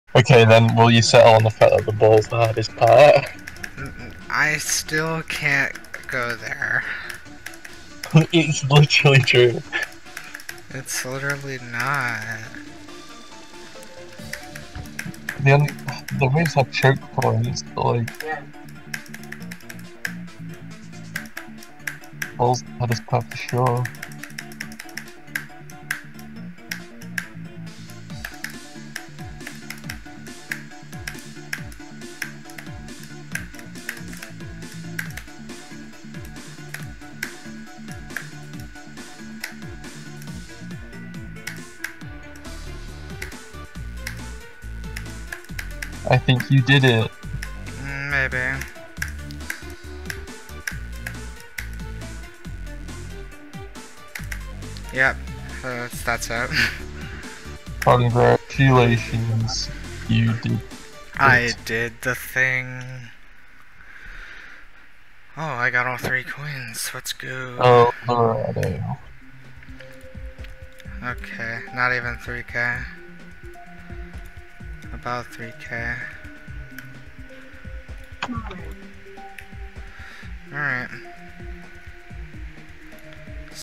El Dorado By LmAnubis 10*
Yet another level that took me 30 years. anyway kinda fun but the first 20% is the hardest part of the level.